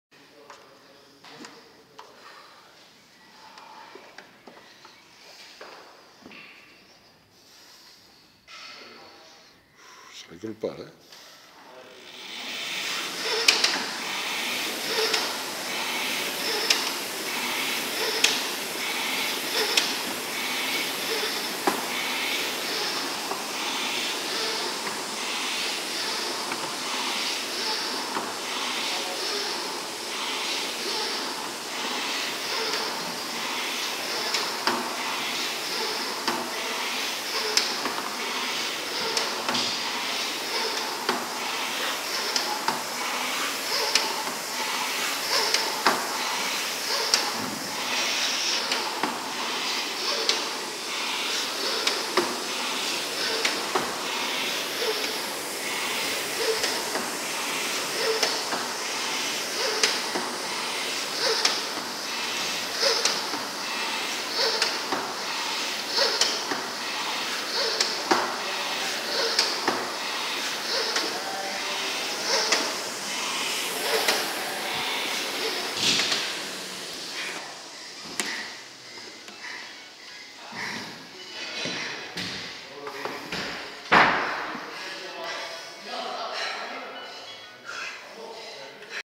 Marilyne prépare le sprint de Port La Nouvelle
Le 18 juin 2016 a lieu le Championnat Open Départemental d'Aviron de Mer, organisé par l'Aviron Nouvellois. Marilyne attaque sa préparation, à La SaLLe,  par des séries de 250m.